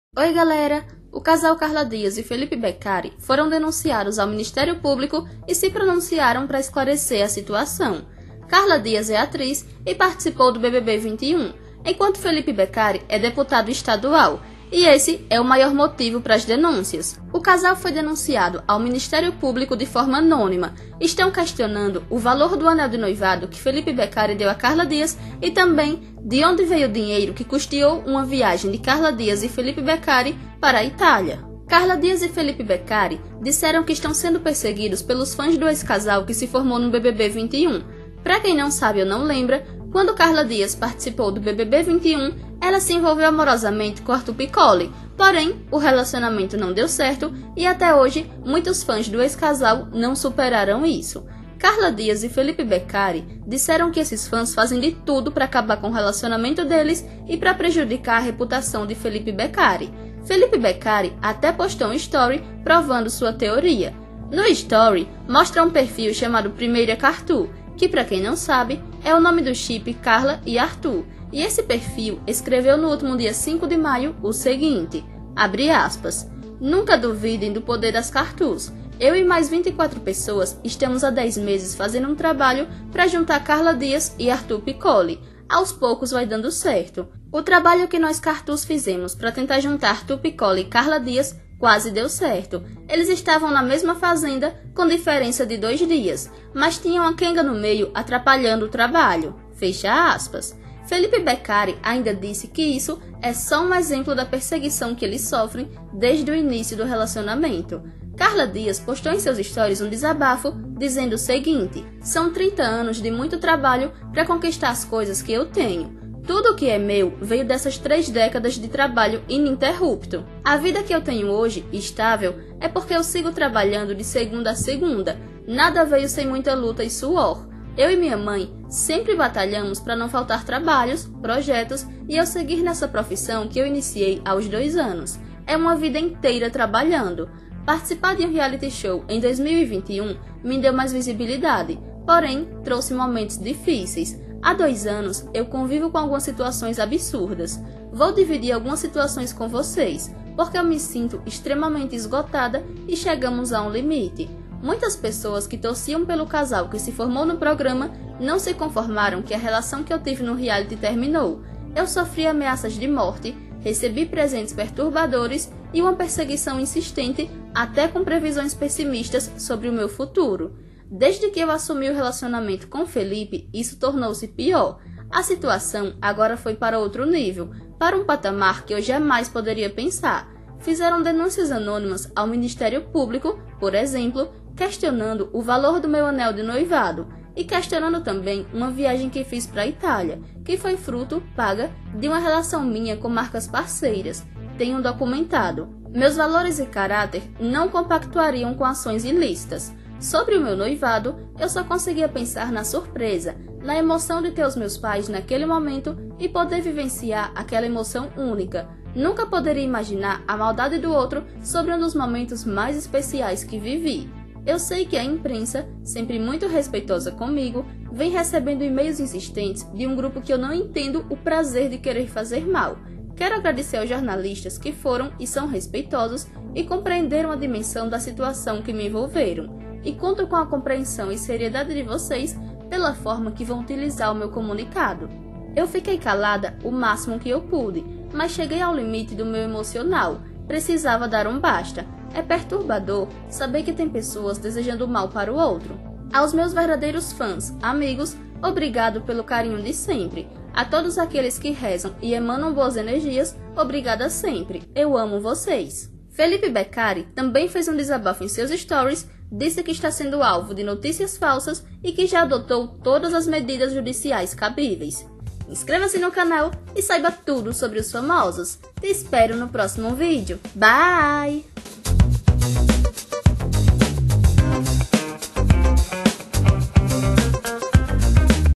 Carla Diaz e Felipe Becari são denunciados ao ministério público e se pronunciam
Carla Diaz e Felipe Becari foram denunciados ao ministério público e se pronunciaram sobre o caso, alegando que as acusações são mentirosas e isso é culpa dos fãs que não superaram o fim do relacionamento de Carla Diaz e Arthur Picoli, que se conheceram no BBB 21.